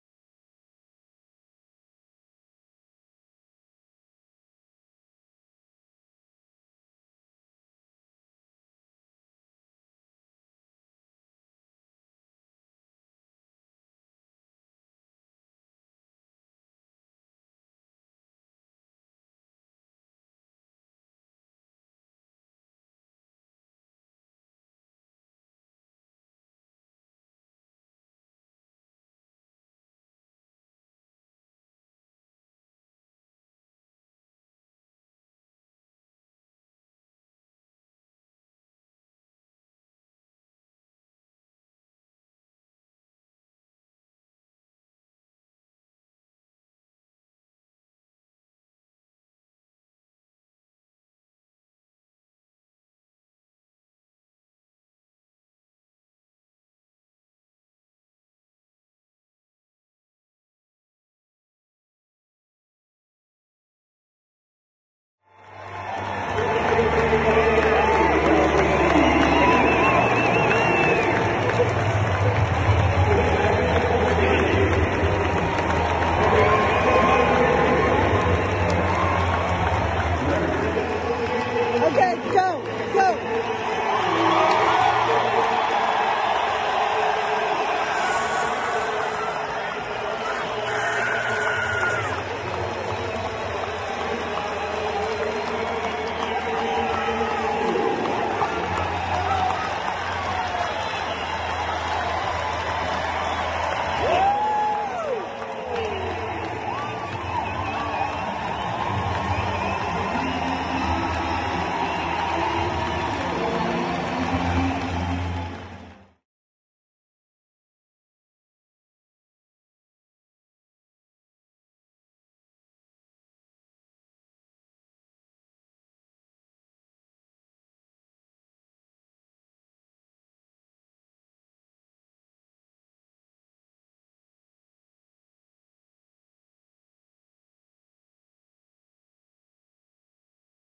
University of Miami Football Game - Veterans Appreciation
The University of Miami honored Veterans on the field during their November 18, 2023 home game. Honored were WWII Veteran, Jack Appel; WWII Veteran, Angelo Demos, and Viet Nam Veteran, Richard White. @MakingHistoryProject